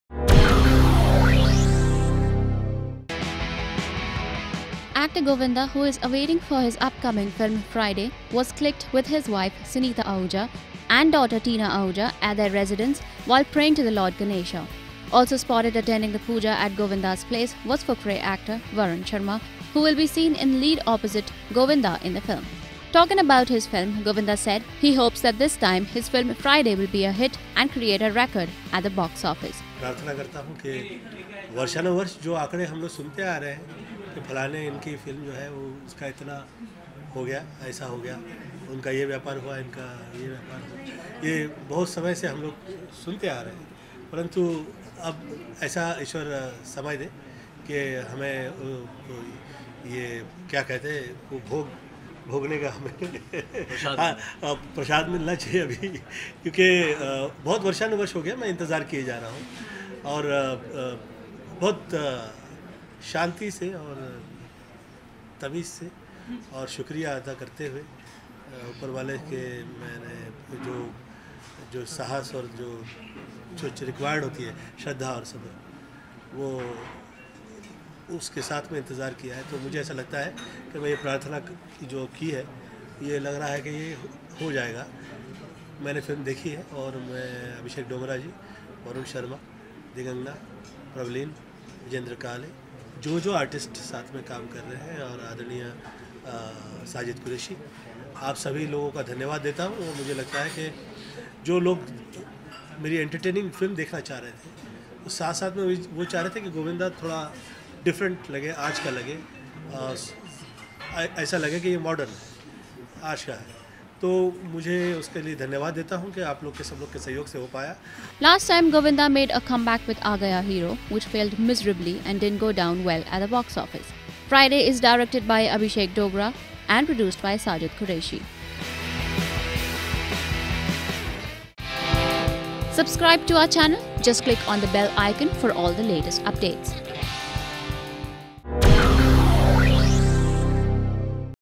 Actor Govinda Prays for “FRY DAY” after "Aa Gaya hero" Debacle
Actor Govinda, who is awaiting of his upcoming film “Fry Day, was clicked with his wife Sunita Ahuja and daughter Tina Ahuja at their residence while praying to the lord Ganesha. Also spotted attending the puja at Govinda’s place was Fukrey actor Varun Sharma, who will be seen in lead opposite Govinda in the film.   Govinda made a comeback with “Aa Gaya hero”, which failed miserably and didn’t go down well at the box office.